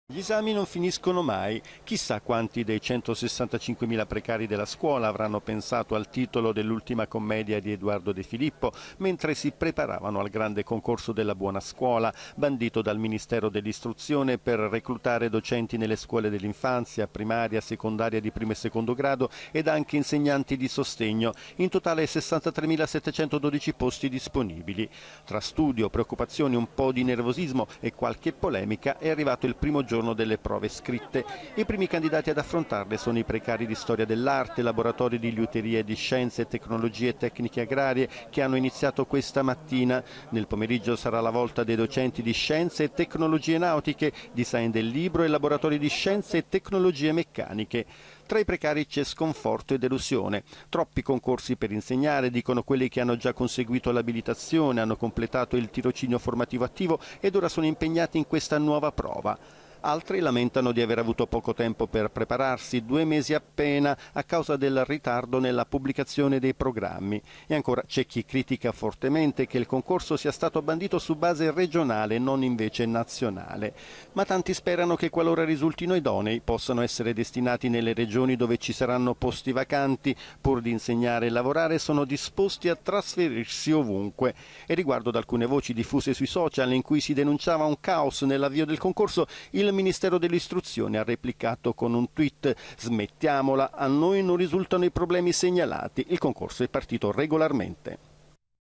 Buona Scuola: al via il mega concorso per insegnanti
Ha preso il via stamattina il mega concorso per insegnanti previsto dalla "buona scuola". 63 mila cattedre per olre 165 mila candidati. intanto, i sindacati della scuola hanno proclamato per il 23 maggio uno sciopero generale. vediamo come sta andando con Francesco Durante